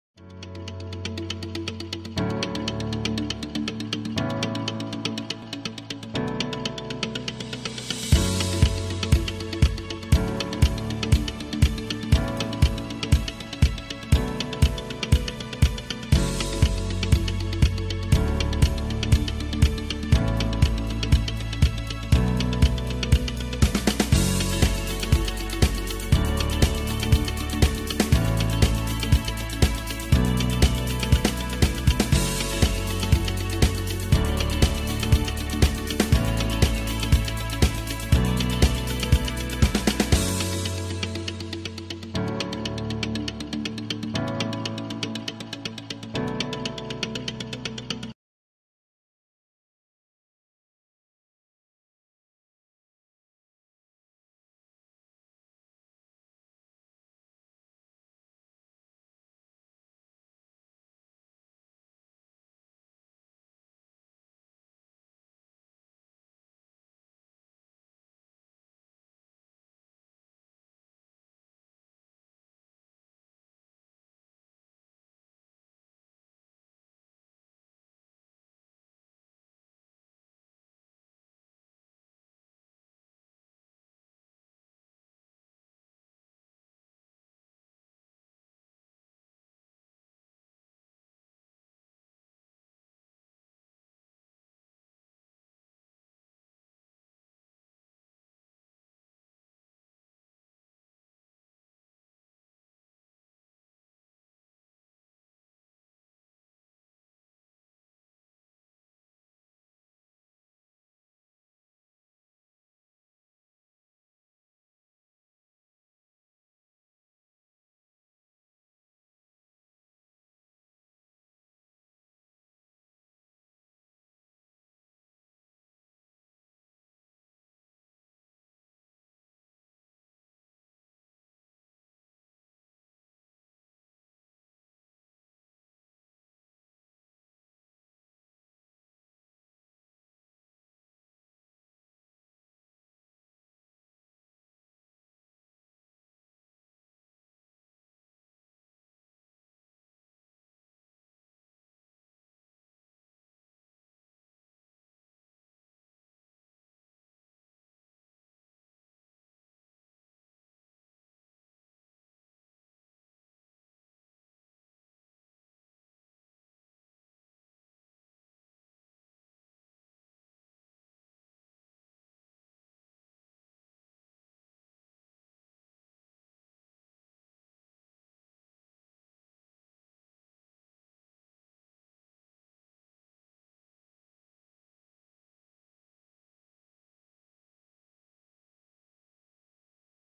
Learn A,B,C.... in fun (pictures) way
Remembering pictures to identify a alphabets letters